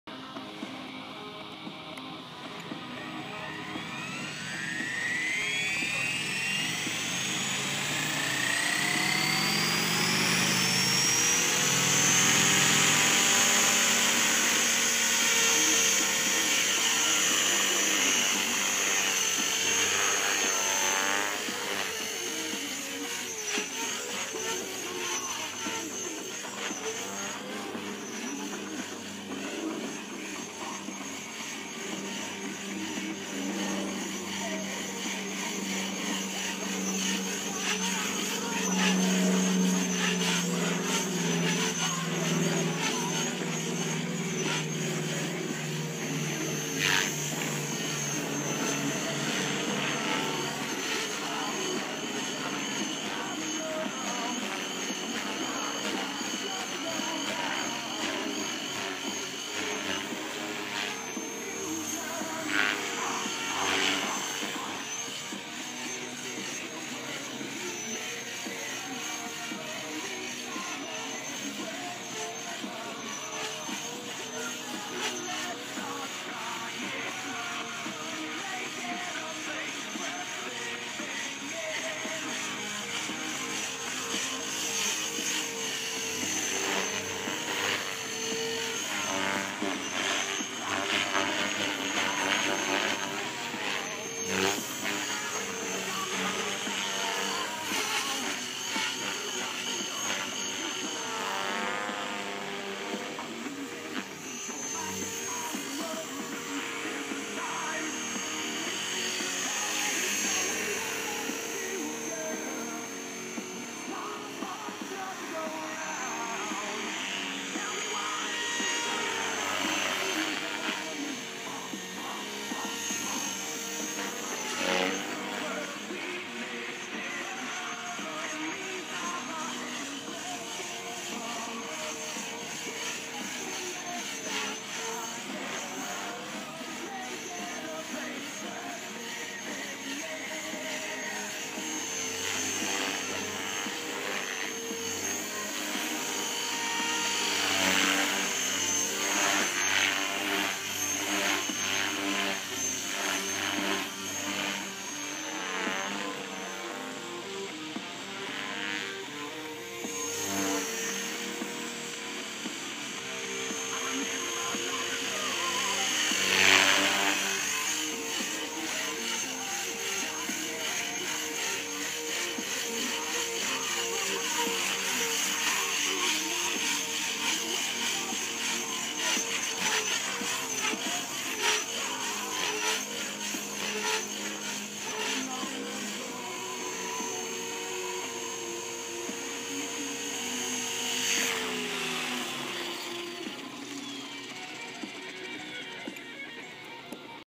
X3 Testing the Align 460MX
Last flight of the day, flying in IU1 using the Governor Store on the YEP 45A Mr HS Calc. 3150RPM